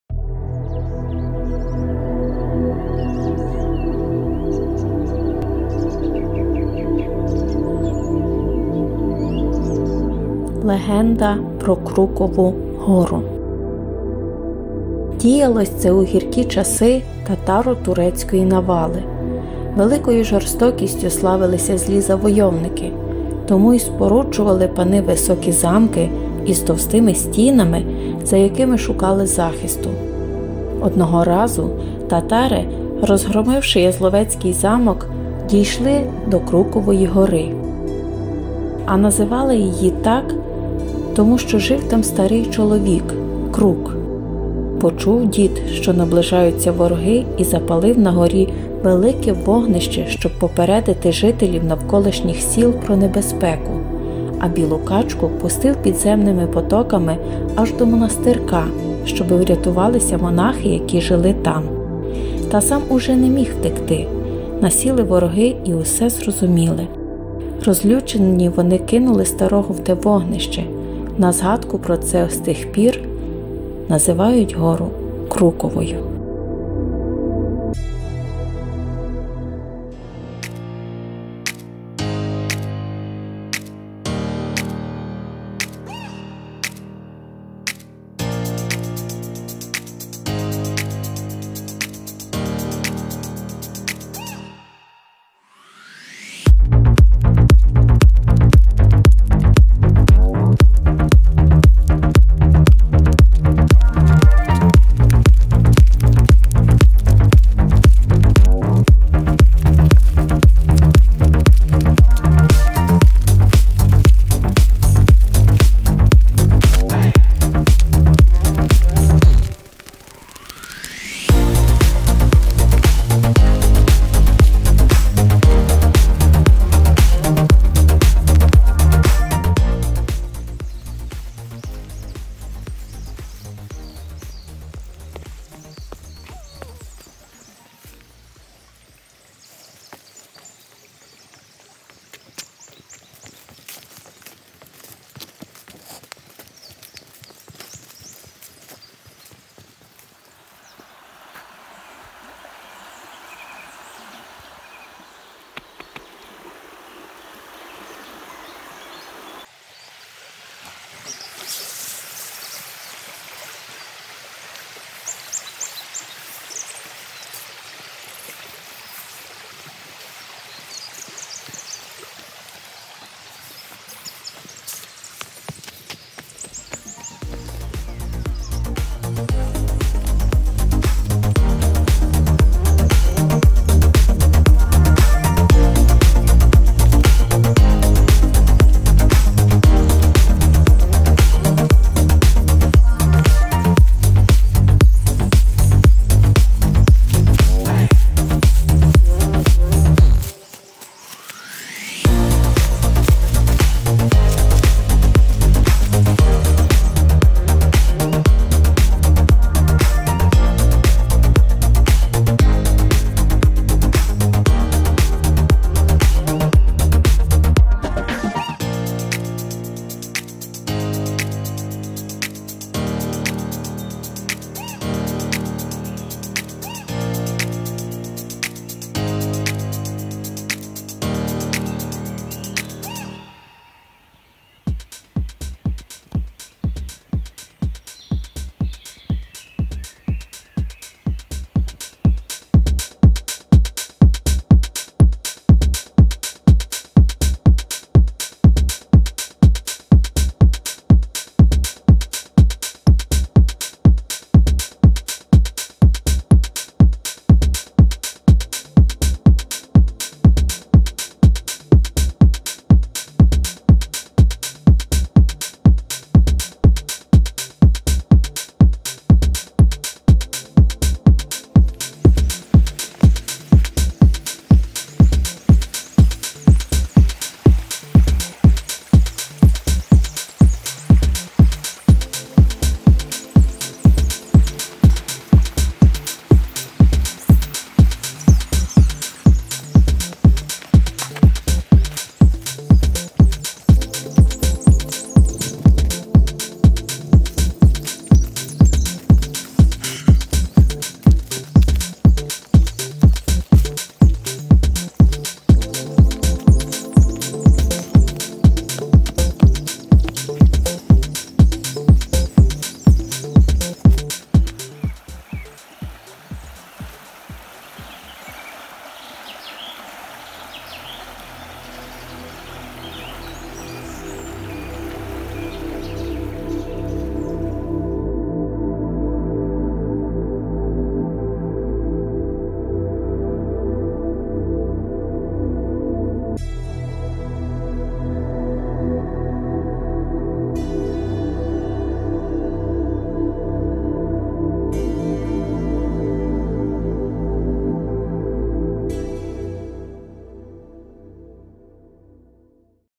Крукова гора
Крукова гора знаходиться в с. Жизномир. Тернопільська обл. Бучацький р-н.